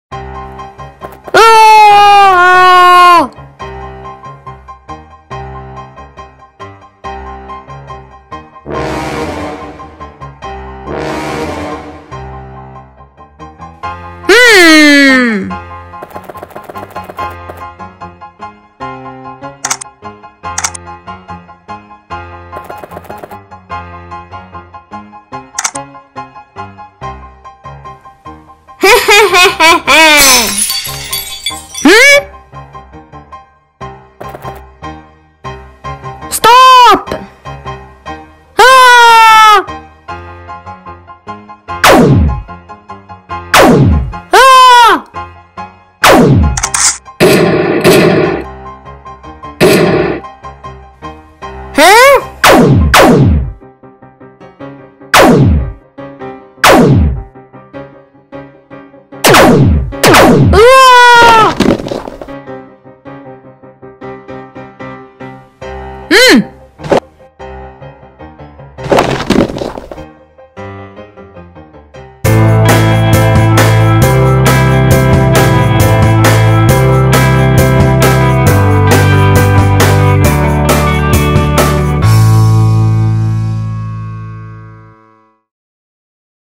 LEGO Prison. Vite, Le Prisonnier S'est échappé! ( Lego Stop Motion )
Animation en 12 images par seconde. Et en Lego stop motion.